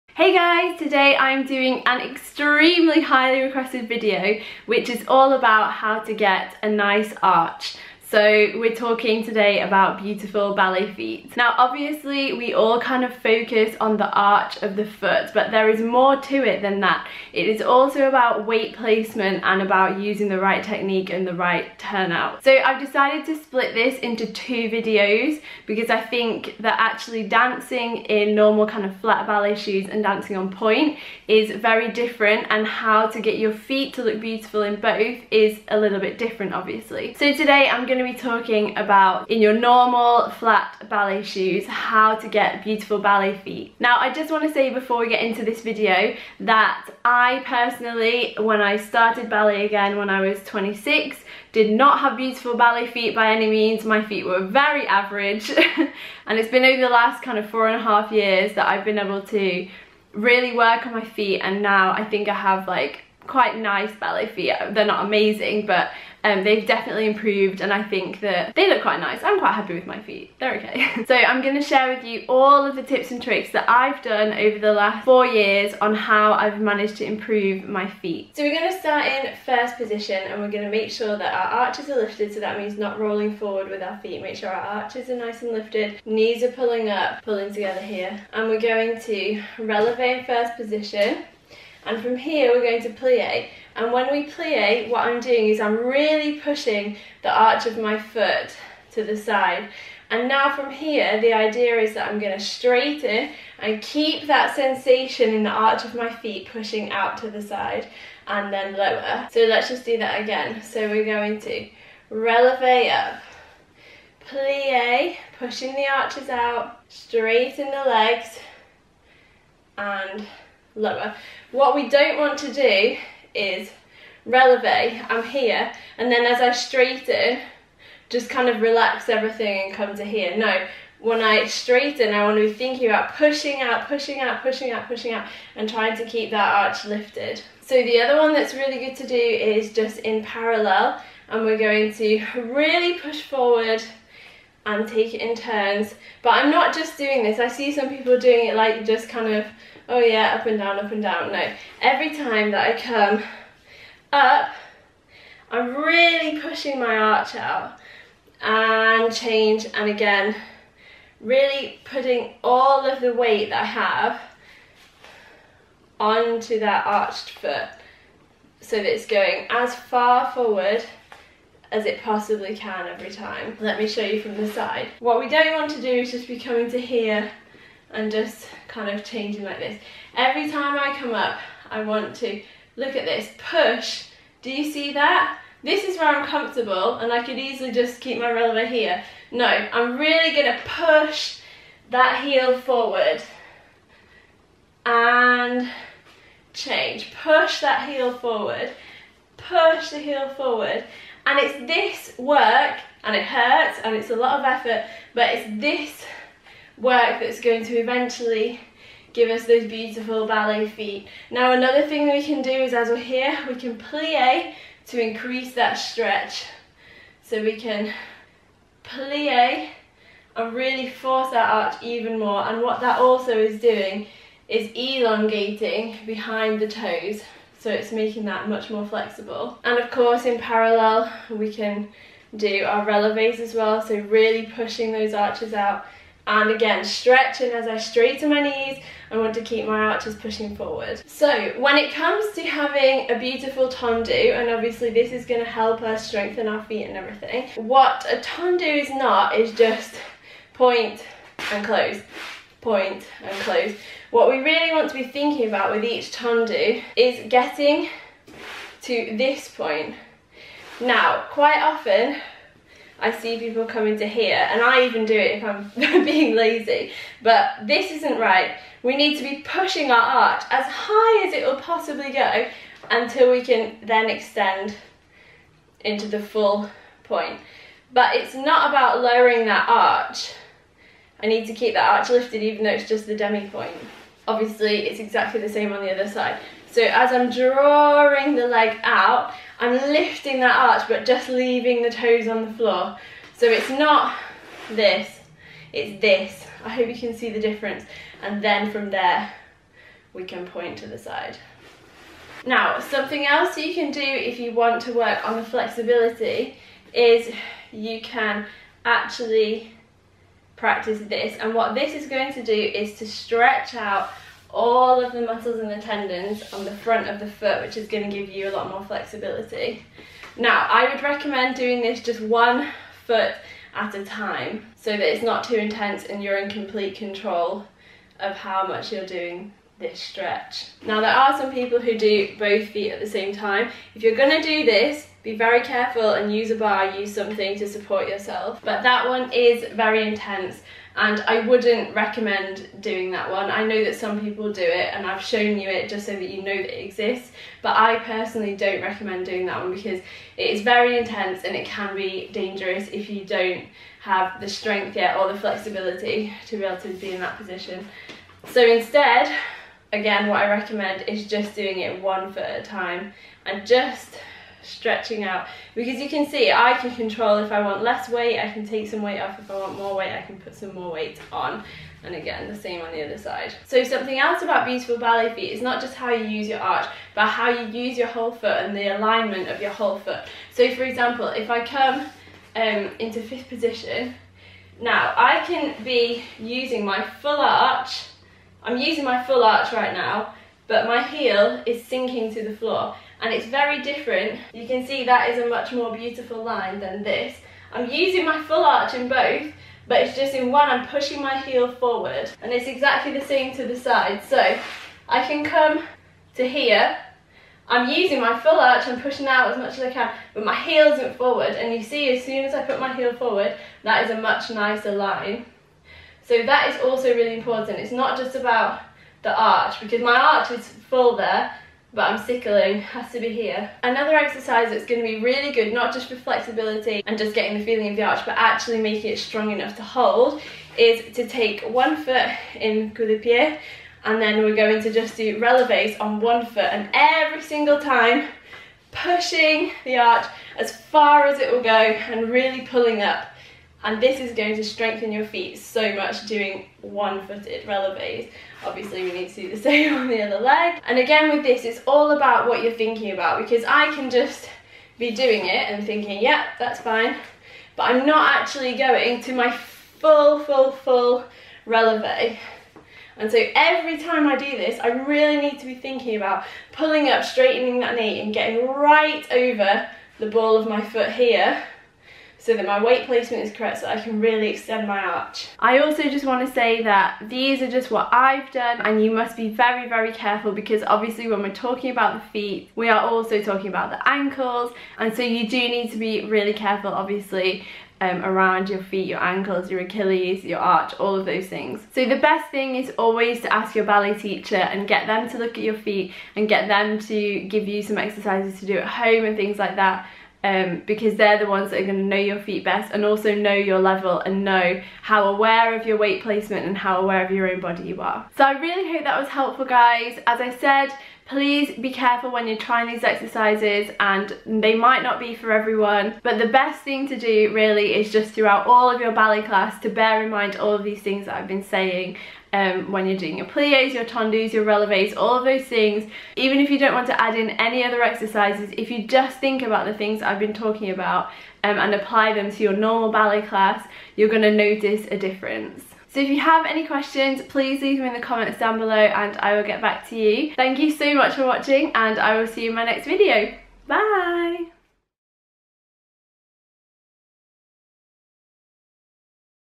HOW TO IMPROVE YOUR ARCH & GET BEAUTIFUL BALLET FEET - Top Tips and Exercises | natalie danza
Want to improve your arches and get ballerina feet? Today I'm sharing my top tips which have really helped me to improve my feet for ballet.

My feet are by no means perfect but they have improved so much since I started ballet nearly 5 years ago and I want to share the things that I do in the hope that it can also help other people.

My Bloch ballet shoes: